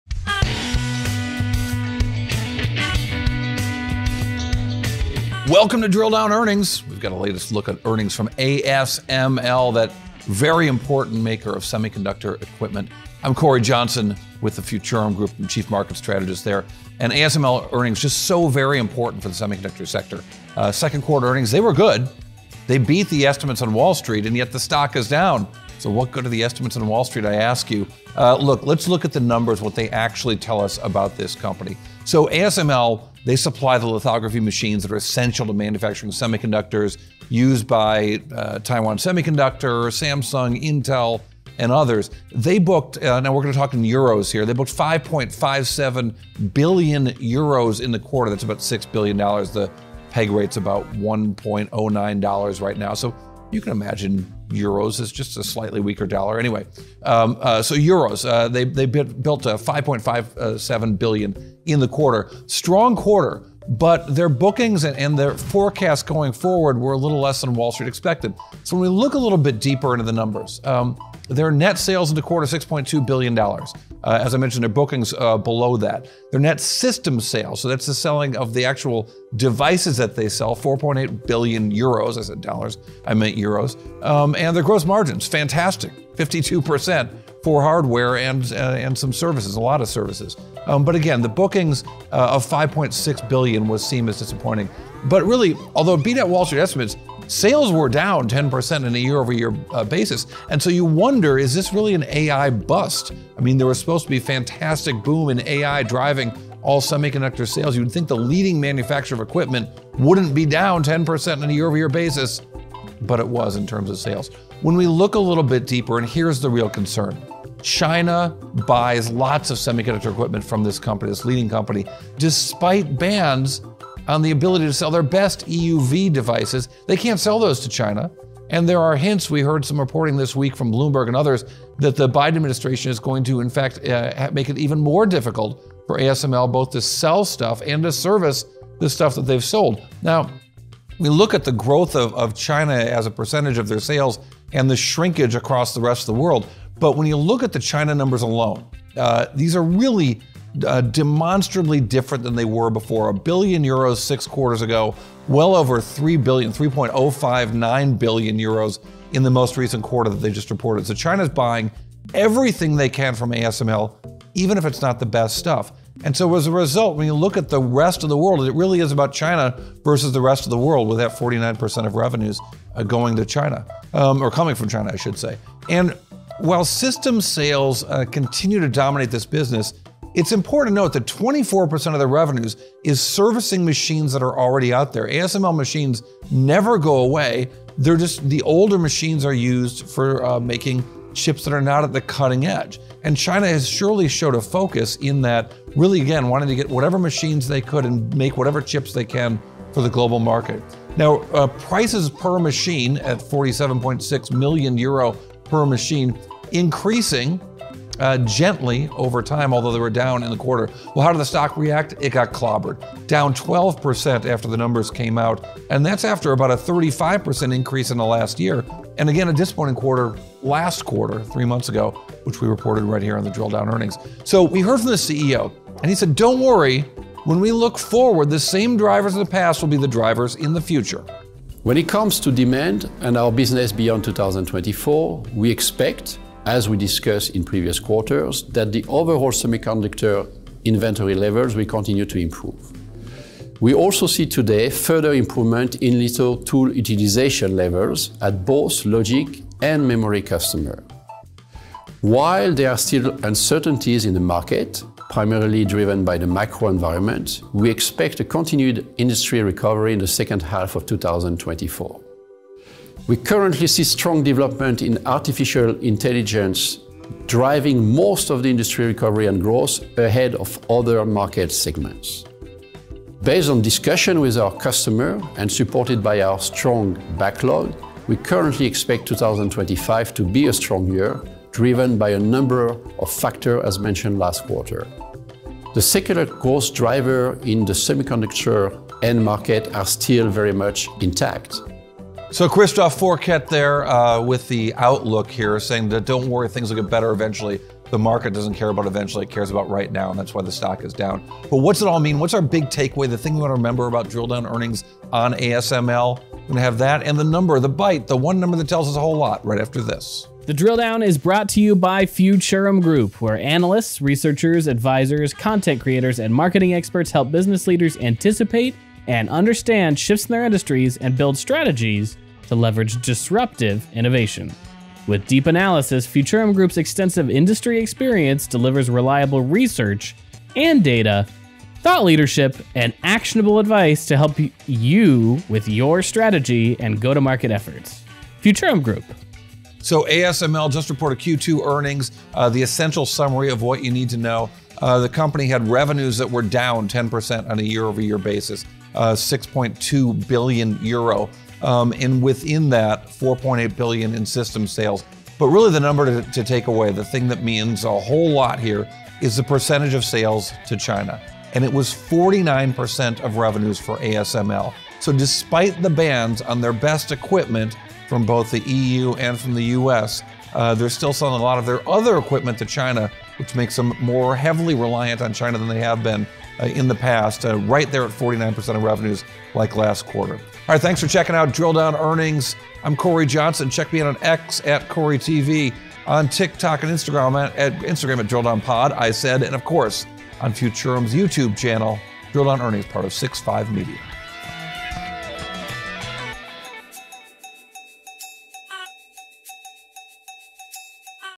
Drill Down Earnings, Ep. 140: ASML Q2 earnings essentials ($ASML)
Instant analysis of ASML ($ASML) Q2 earnings, and we hear directly from CEO Christophe Forquet.